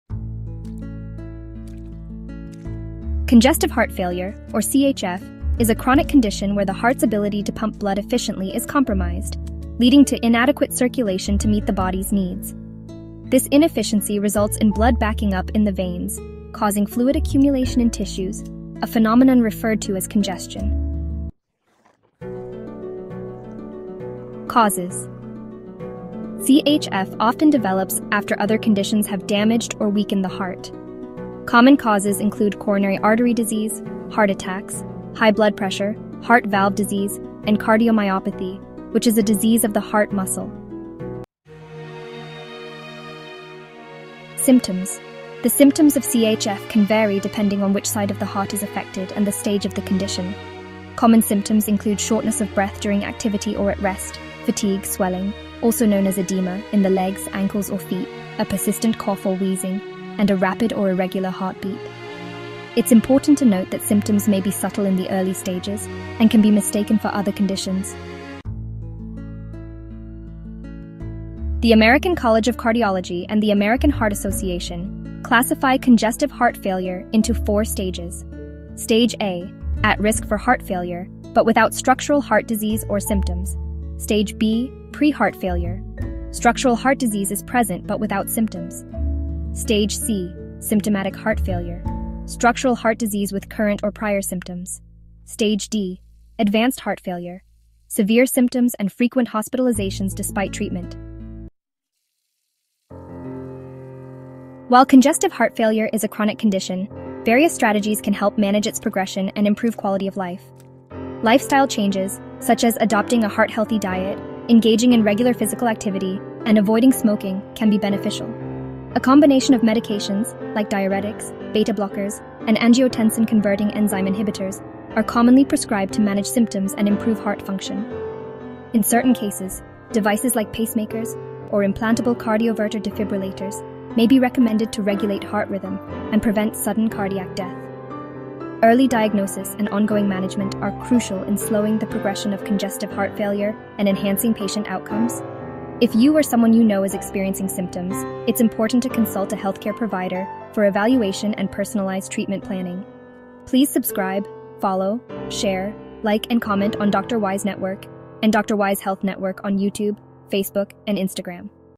What Is CHF Doing to Your Body? Understanding Congestive Heart Failure.
Congestive heart failure (CHF), commonly referred to as heart failure, is a chronic condition in which the heart is unable to pump blood effectively to meet the body’s needs.